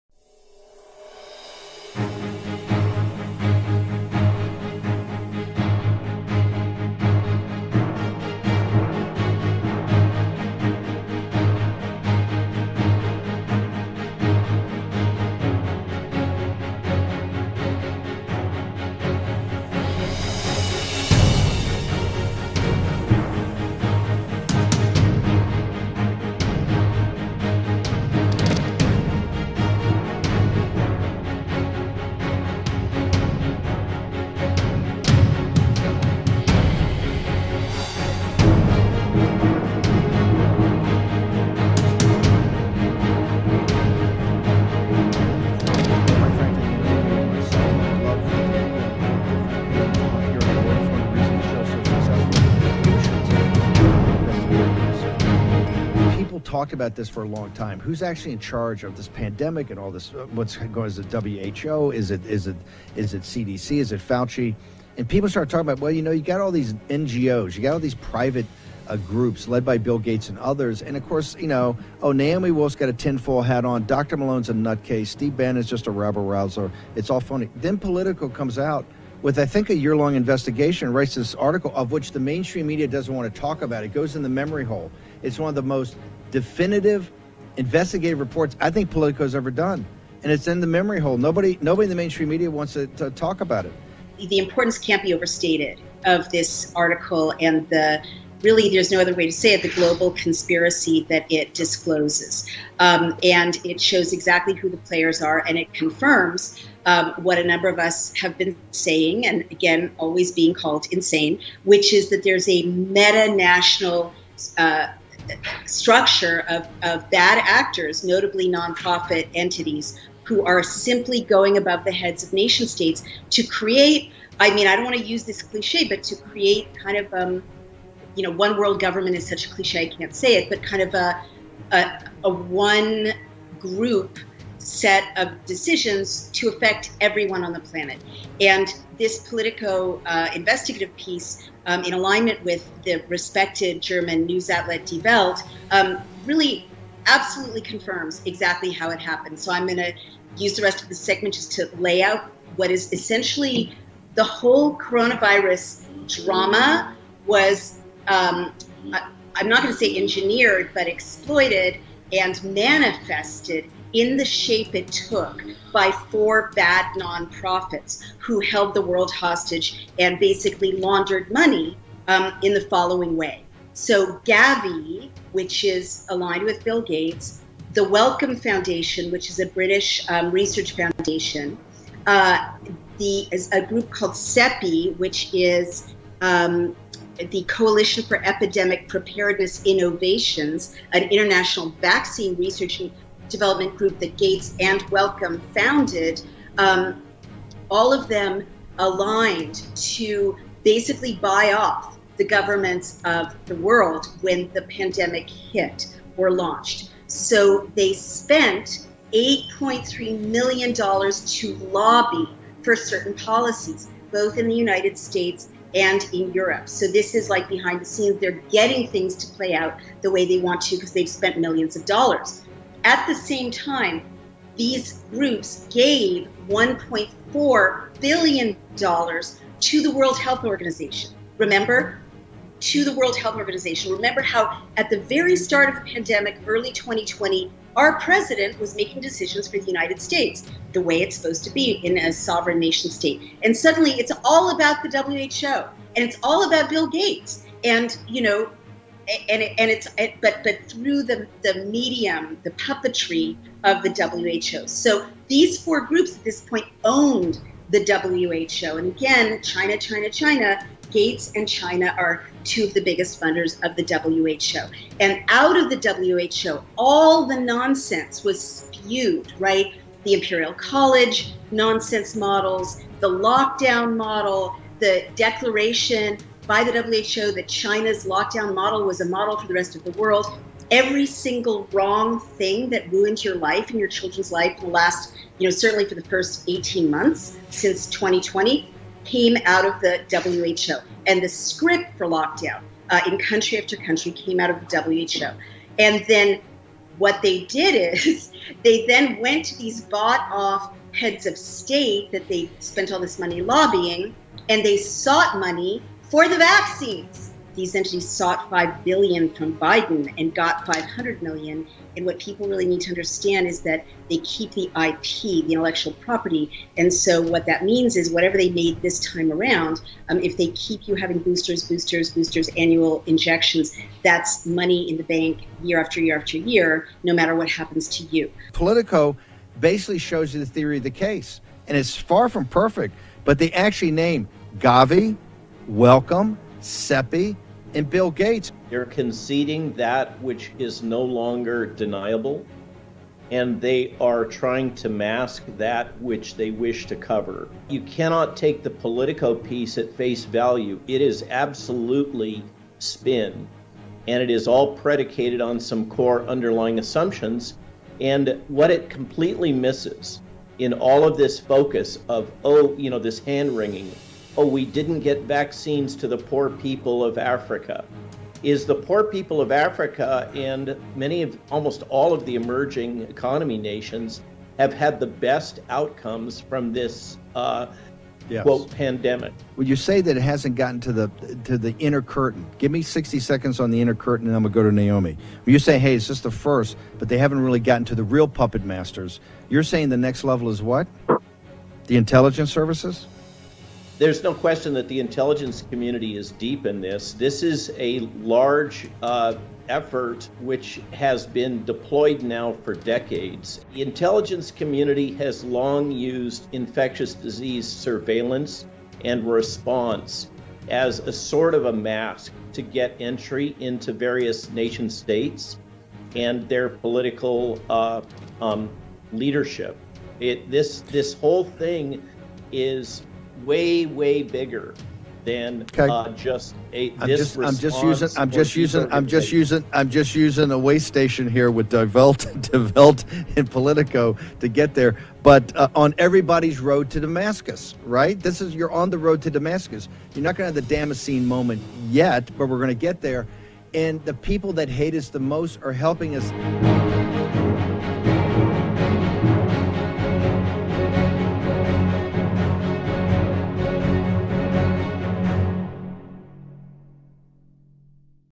Wendet sich der Mainstream jetzt gegen Bill Gates
‼️ Wendet sich der Mainstream jetzt gegen Bill Gates? “Welt” und “Politico” decken Corona-Machenschaften auf

Was bis vor Kurzem noch als Verschwörungstheorie galt, bestätigt nun der Mainstream! Sechs Monate lang recherchierten “Welt” und “Politico” auf beiden Seiten des Atlantiks zur Rolle von Bill Gates in der Corona-Krise.

In einem Bericht vom 19. September resümieren sie: “Wichtige Entscheidungen trafen nicht die Staatschefs und die Weltgesundheitsorganisation, sondern die Stiftung von Bill und Melinda Gates und deren Netzwerk.”

Die erstaunlich deutlichen Recherchen werden im Mainstream bislang so gut wie totgeschwiegen.

+++ Bitte diesen aufsehenerregenden Beitrag großflächig teilen ... +++